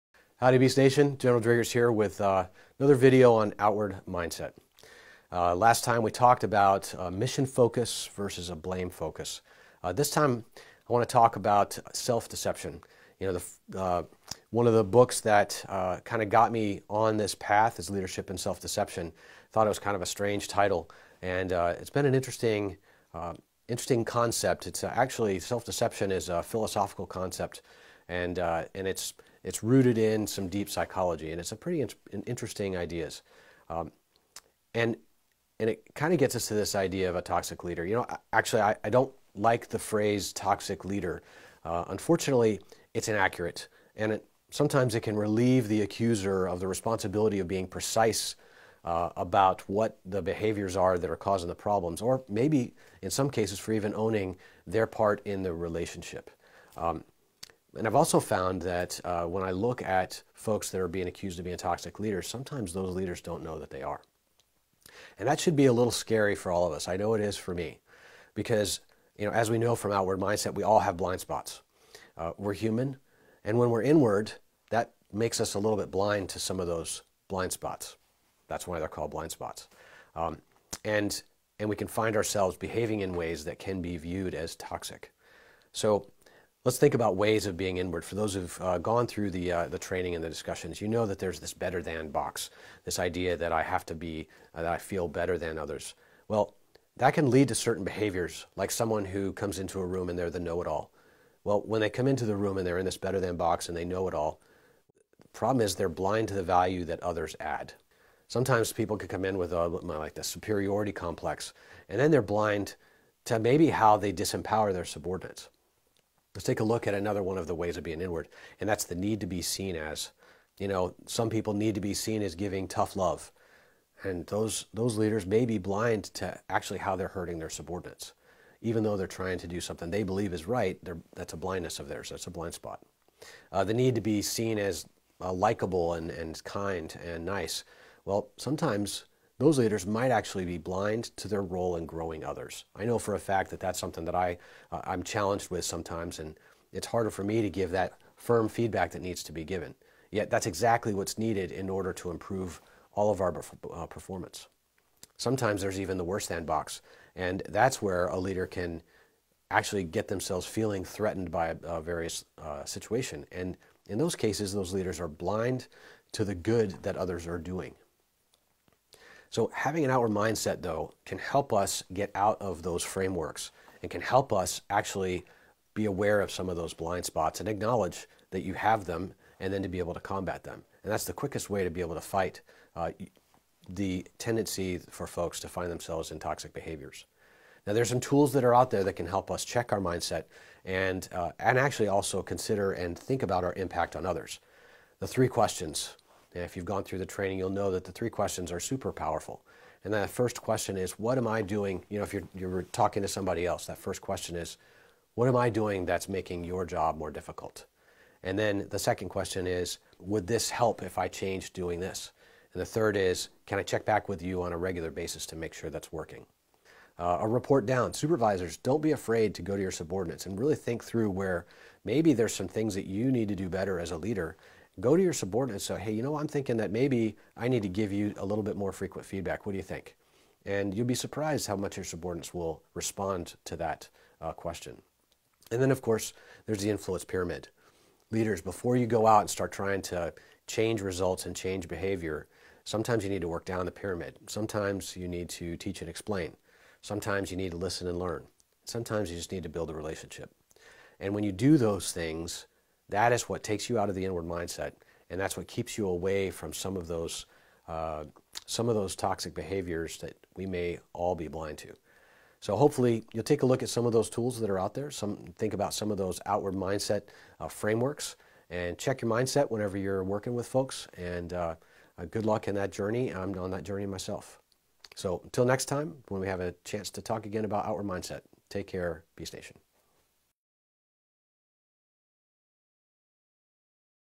Outward Mindset #3
🌟 The Power of Outward Mindset 🌟

In this episode, Brig. Gen. Driggers delves into the critical topic of leadership and self-deception. Join us as he explores the detrimental behaviors that often undermine effective leadership. Gain valuable insights to elevate your leadership skills and make a positive impact.